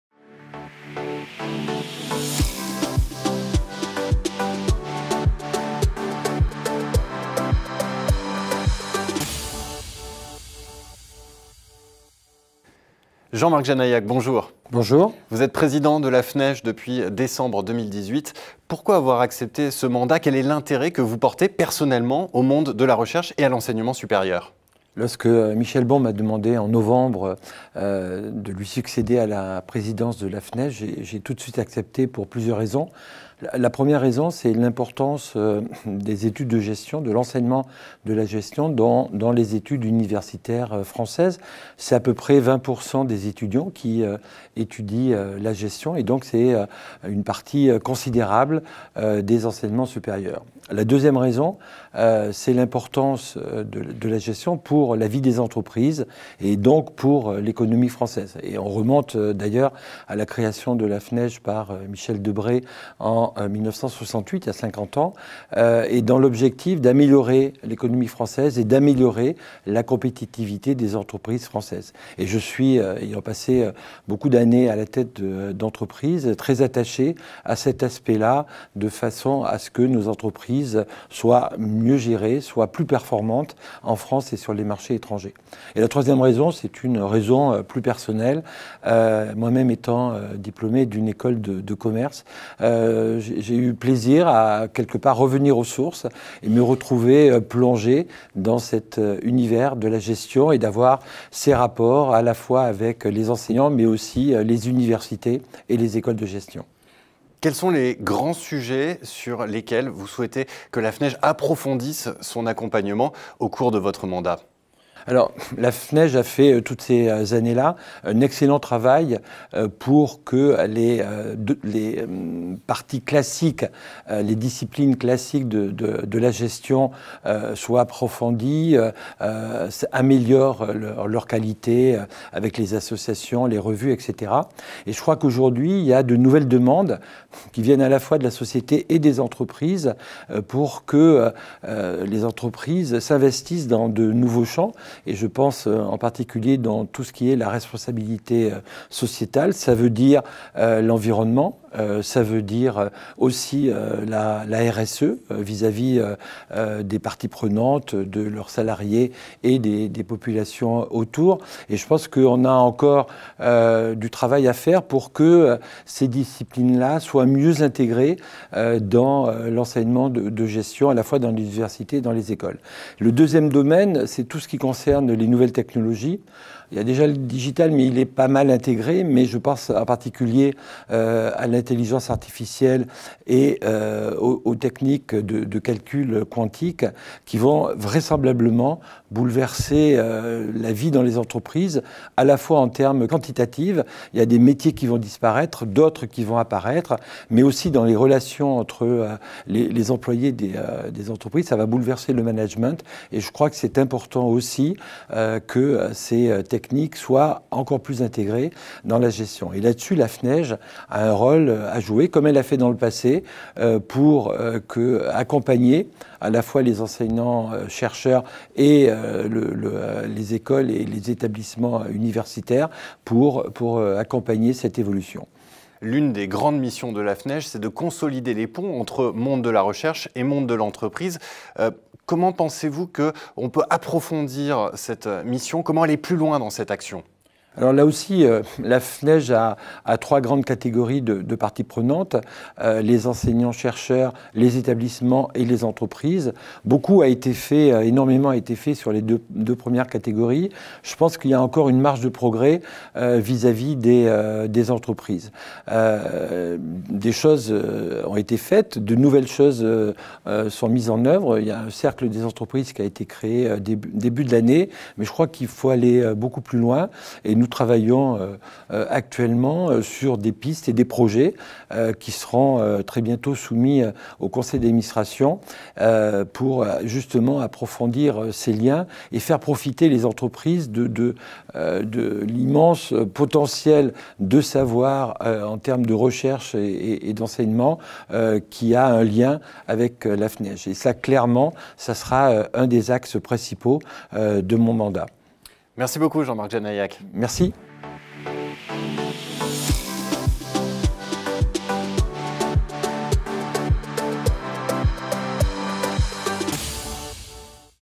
Rôle de la gestion, RSE, IA… Jean Marc JANAILLAC se confie sur ses missions de Président de la FNEGE
Jean-Marc JANAILLAC, Président de la FNEGE, s’entretient en vidéo avec Thibault LIEURADE pour échanger sur les missions de son mandat à la Fondation. Le Président revient tout d’abord sur les différentes raisons qui l’ont motivé à accepter cette mission : la première est l’importance des études de gestion dans la vie universitaire en France (20% des étudiants font partie d’un cursus de gestion).
La deuxième raison est son attachement pour l’importance de la gestion dans la vie des entreprises, partie intégrante de l’économie française. Enfin, il nous confie que c’est aussi pour lui l’occasion de faire un retour aux sources, ayant fait des études de commerce et de gestion. Concernant ces missions futures à la FNEGE, Le Président souhaite se concentrer sur les nouvelles demandes des entreprises en termes de gestion environnementale, de RSE, mais aussi de technologies poussées (IA) pour permettre, grâce au monde académique, d’accompagner les entreprises dans ces mutations et ainsi faire le pont entre ces deux parties prenantes grâce à la Fondation.

Nom(s): LIEURADE Thibault; JANAILLAC Jean-Marc
Institution(s); The Conversation France; FNEGE;
Fonction(s): Président